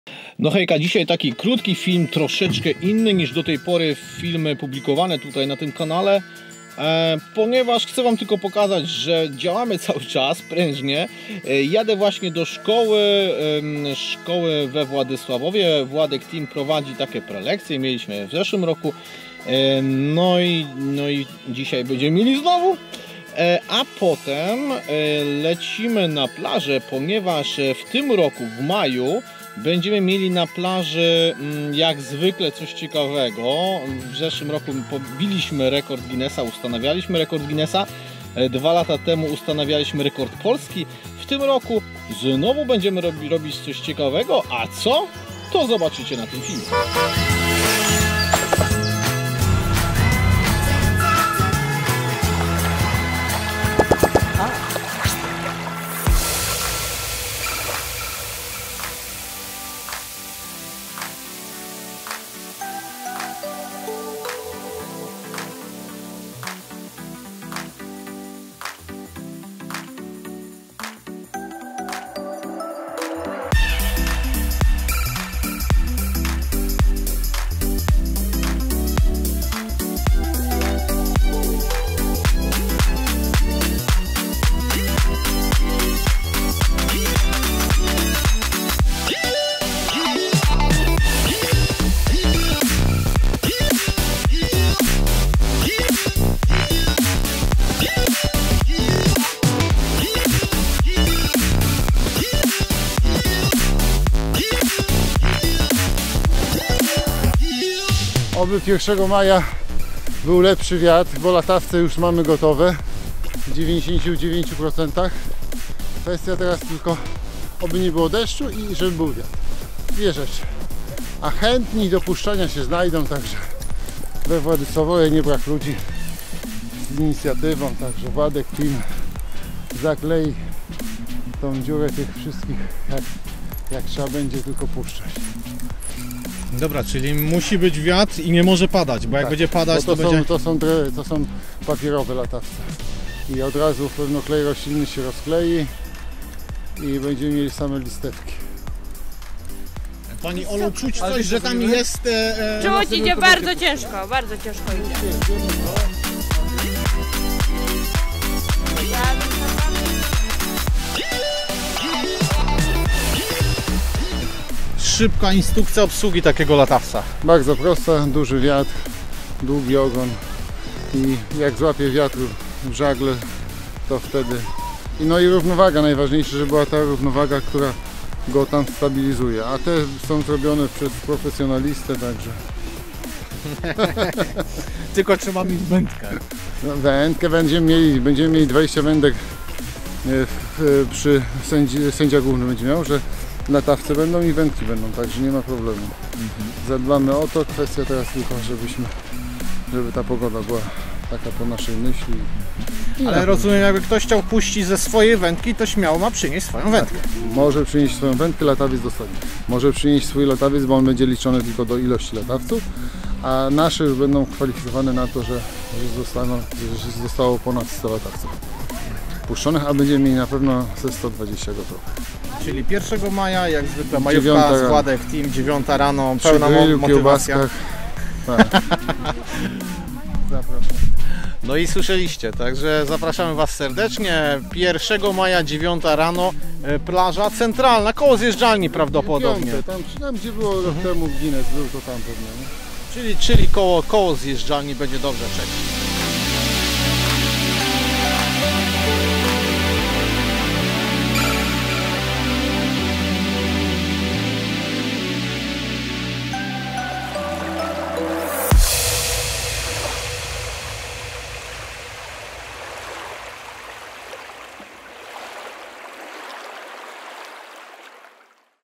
Przygotowania do Rekordu Polski + lekcje w szkole...
To NIE JEST ta oczekiwana przez Was NOWA SERIA - film pokazuje dzień z mojego życia :) A dzieje się u minie dużo :) Także tak wyglądał piątek a w sobotę znów jestem we Władku bo mamy spotkanie w sprawie planowanych imprez na ten rok... Pozdrawiam